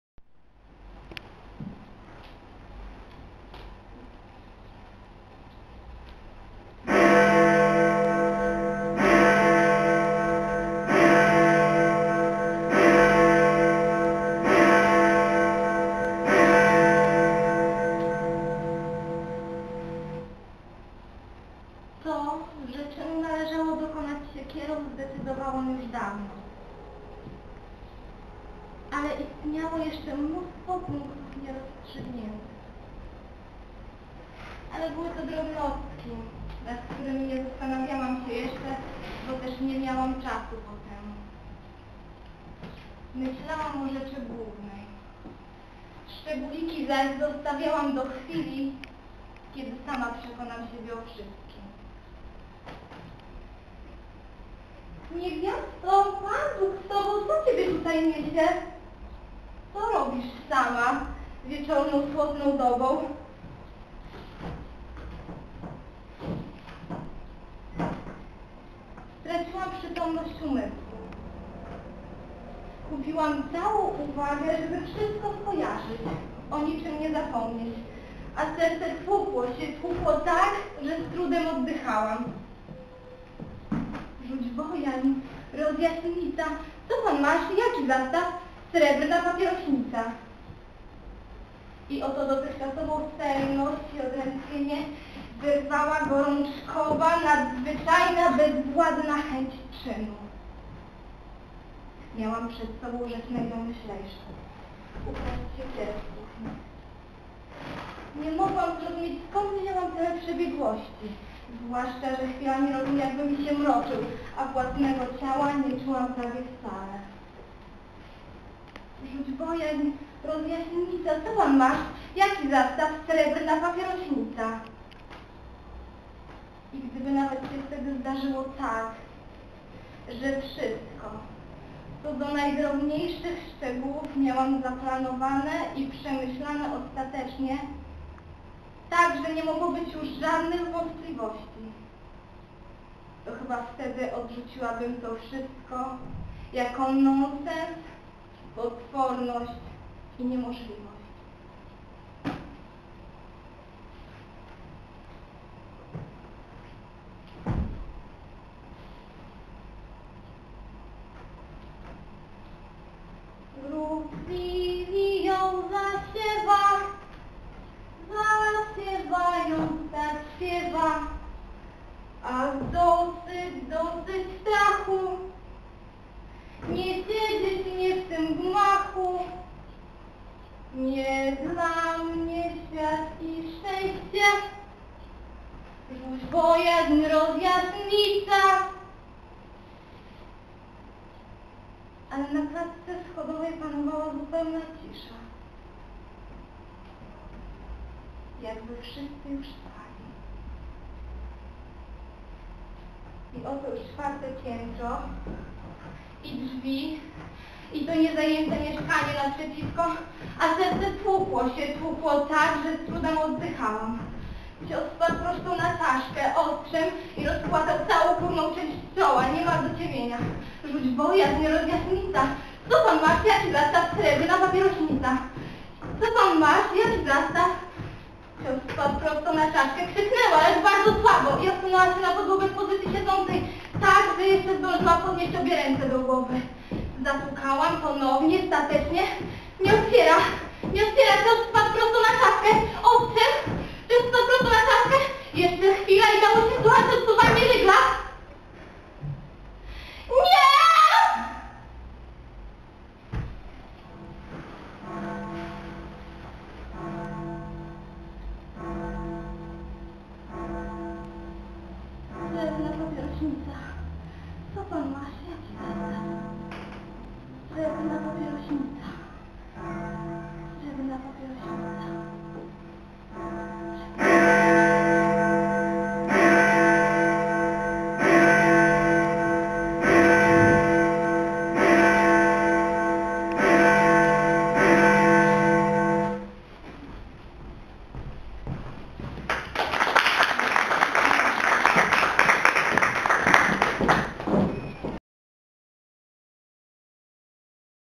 zbrodnia
Minimonodram - wywiedzione ze słowa na podstawie "Zbrodni i kary" F. Dostojewskiego i ballady "Lilije" A. Mickiewicza. Występuje: Ewelina Zaremba. Scenariusz i reżyseria: Dariusz Parczewski. Produkcja: Teatr 3/4 i Katolickie Liceum Ogólnokształcące w Białej Podlaskie w 2005 roku. Główae nagroda w 50. Ogólnopolskim Konkursie Recytatorskim im. Adama Mickiewicza w Białej Podlaskiej i Lublinie. Finał w Teatrze Rondo w Słupsku.  realizacja: Dariusz Parczewski
Lublin - Biala Podaska RP 2005